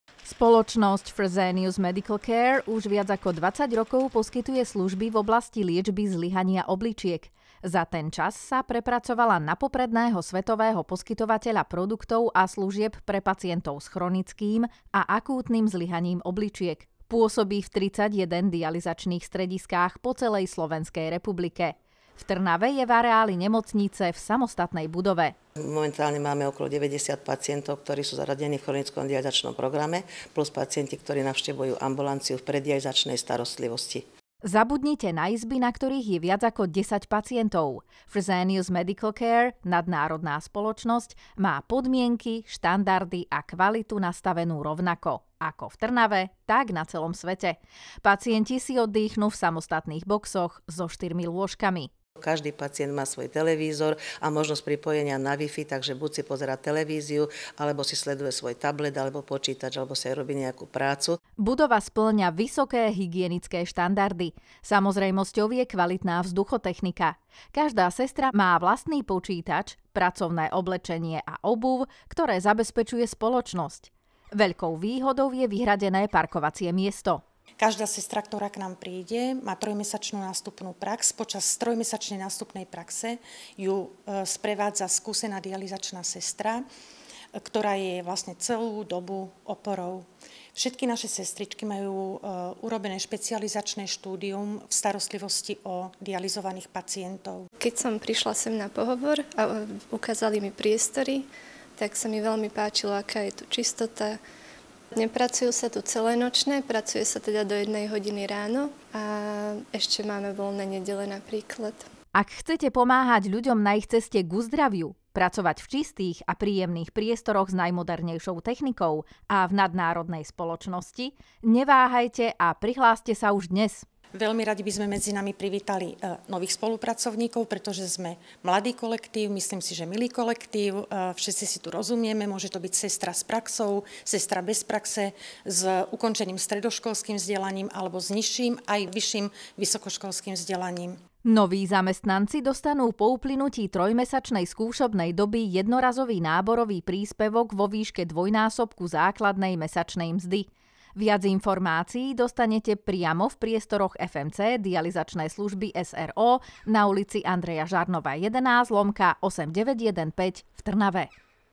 Dialyzačné stredisko v Trnave
Máte zdravotnícke vzdelanie a chcete pracovať v nadnárodnej firme, v čistom prostredí a s najmodernejšími technológiami? V Dialyzačnom stredisku v Trnave hľadajú práve vás.